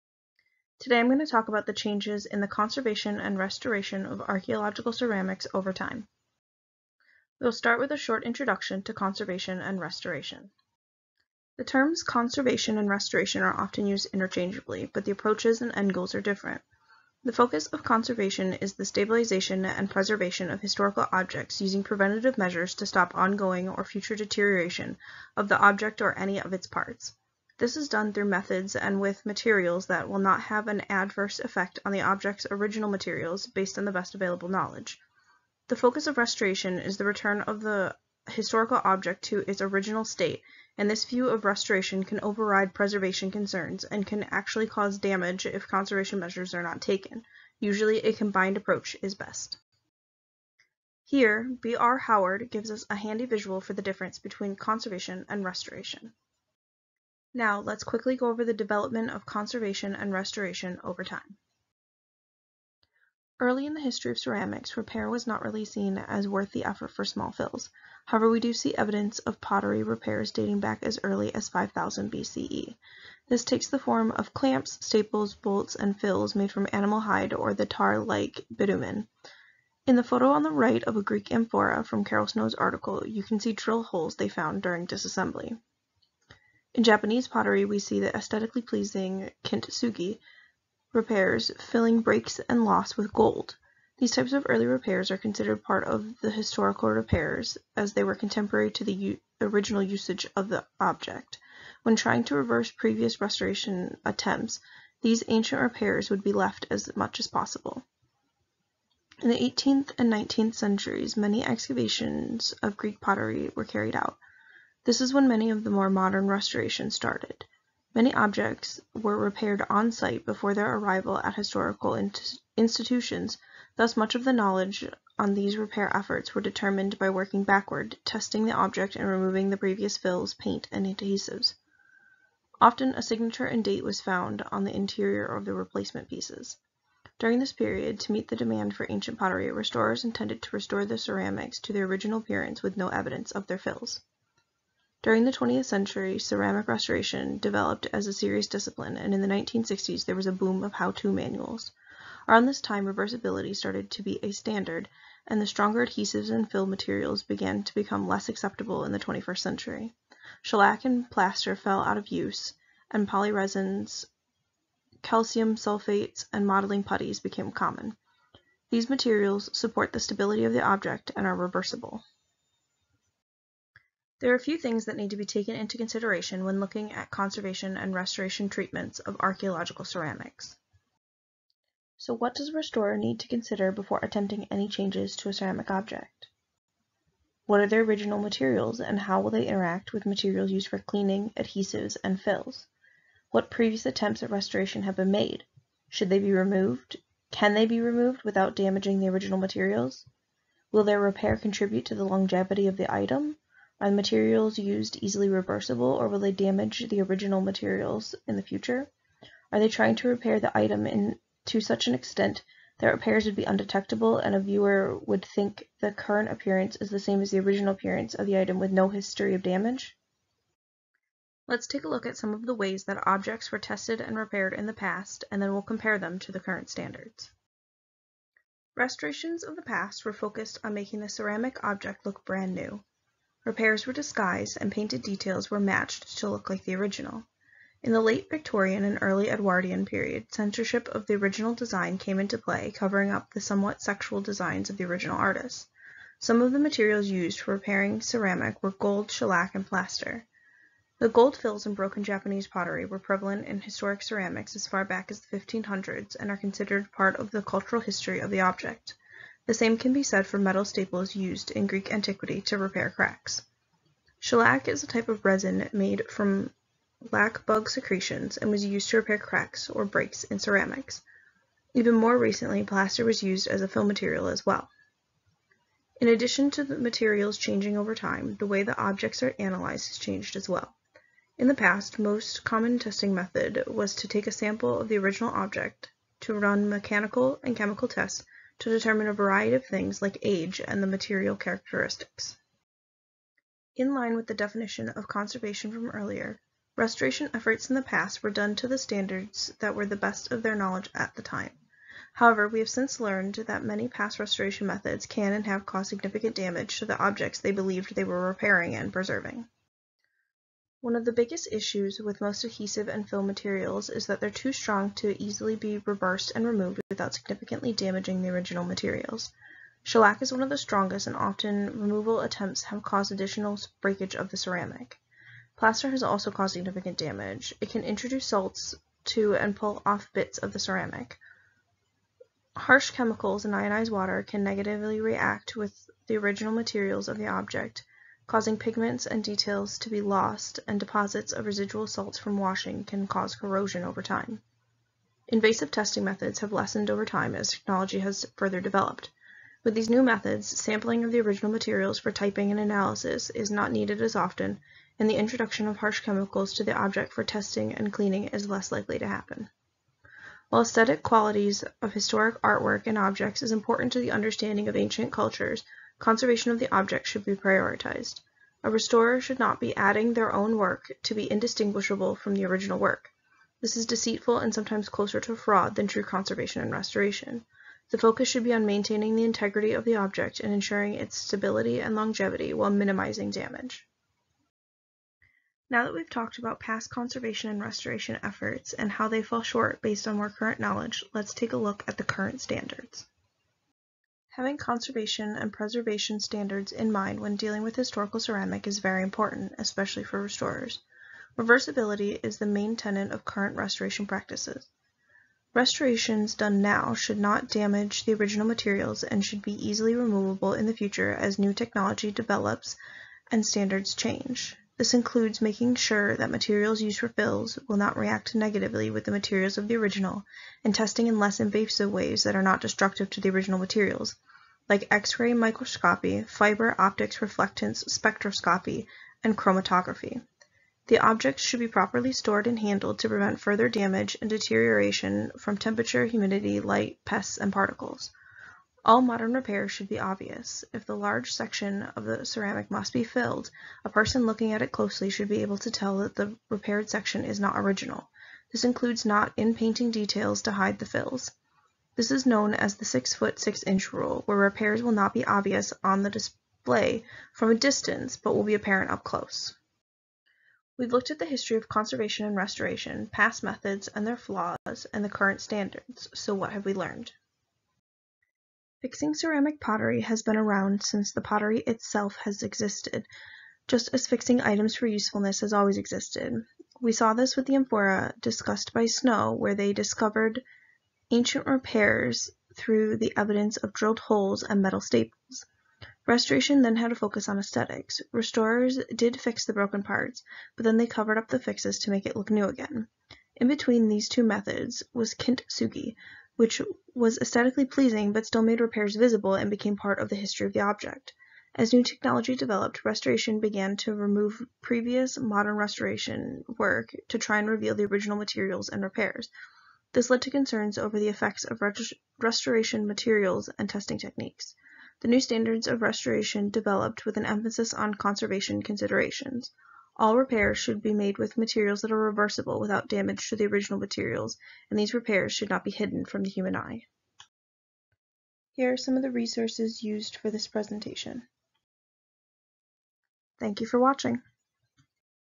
Changes in Conservation & Restoration of Archaeological Ceramic
Final Project for INF 7750 at Wayne State University.

Copyright Rachel Smith, 2020.